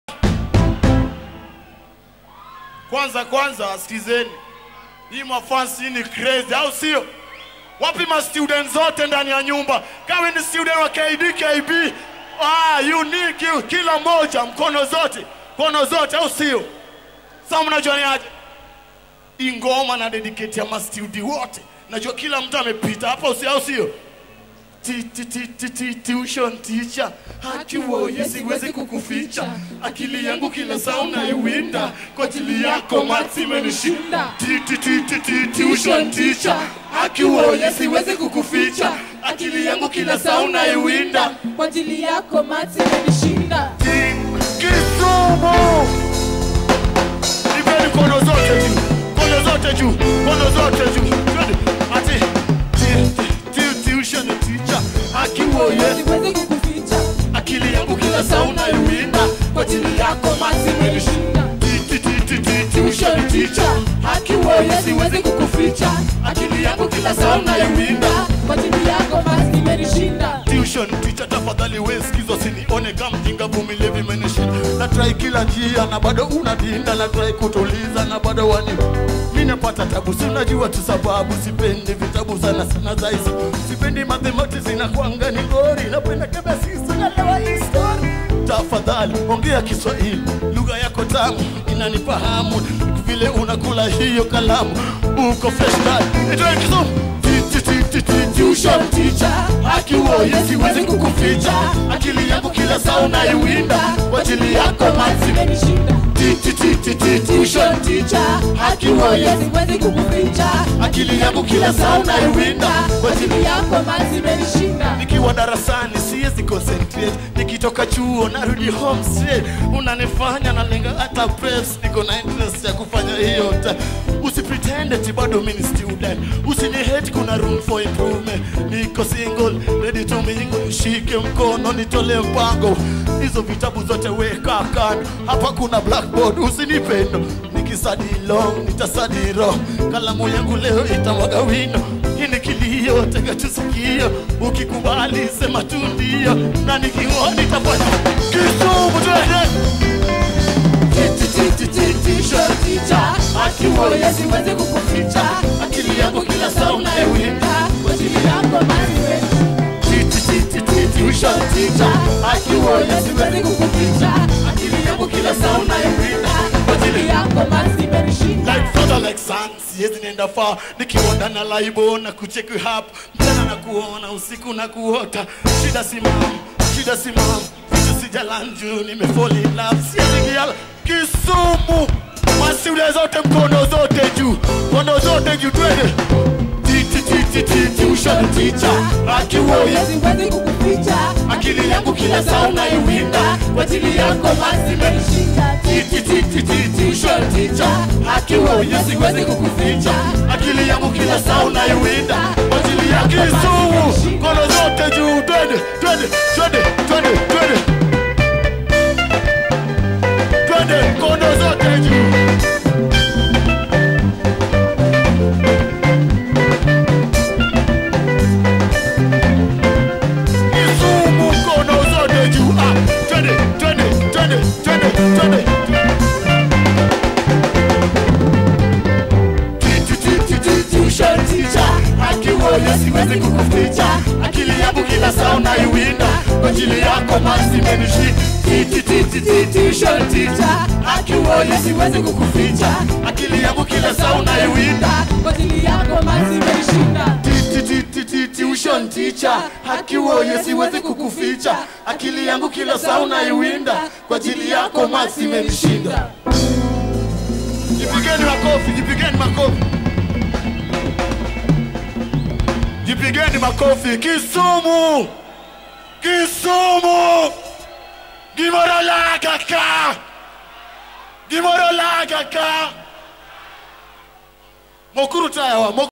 Safaricom Kenya Live Concert Kisumu -Nebulazz
The Safaricom Kenya Live Concert in Kisumu- Nebulazz performance.Kisumu guys came out in large numbers to see Nebulazz amongst other artists like Redsan, Wyre,Wahu,Ekodydda and Osogo Winyo.